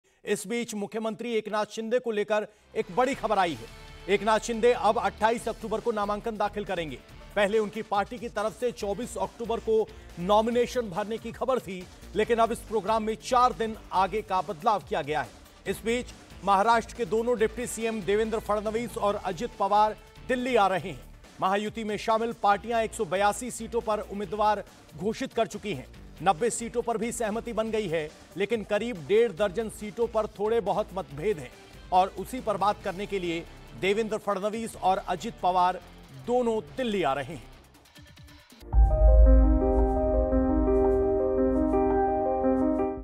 Breaking News: एकनाथ शिंदे ने अपने नामांकन की तारीख बदली, इस तारीख को करेंगे नामांकन | Shivsena
एकनाथ शिंदे अब 28 अक्टूबर को नामांकन भरेंगे...पहले उनकी पार्टी की तरफ से 24 अक्टूबर को नॉमिनेशन भरने की खबर थी लेकिन अब इस प्रोग्राम को चार दिन आगे कर दिया गया है..इस बीच महाराष्ट्र के दोनों डिप्टी सीएम देवेंद्र फडणवीस और अजित पवार दिल्ली आ रहे हैं..